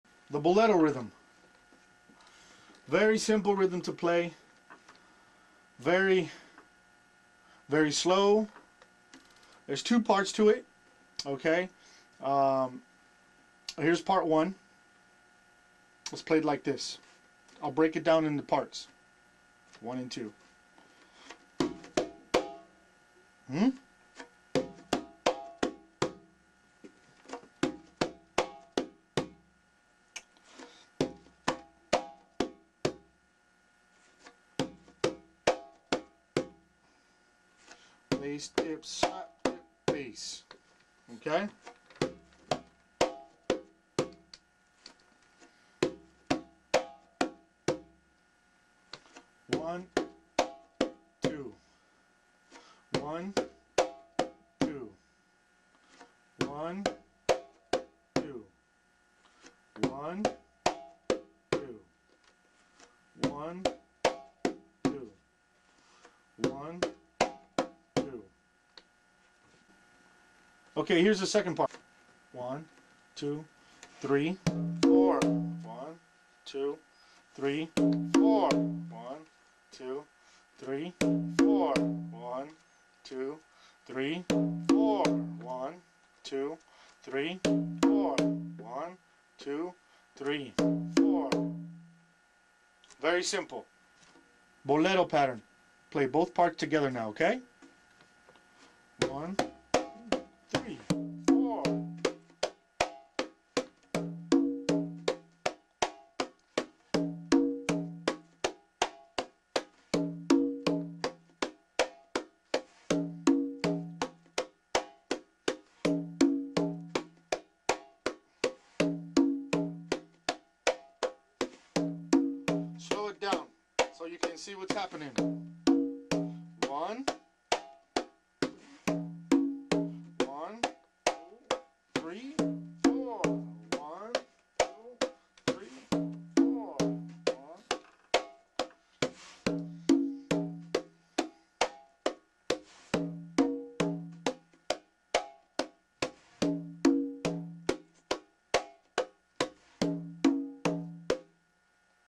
Bolero - First Rhythm for Beginners - Rudy Quesada
This is a very simple rhythm for beginners who are just starting to learn their first rhythm. The reason I say this one first is because it's very basic and easy to pick up from the way I demonstrate it.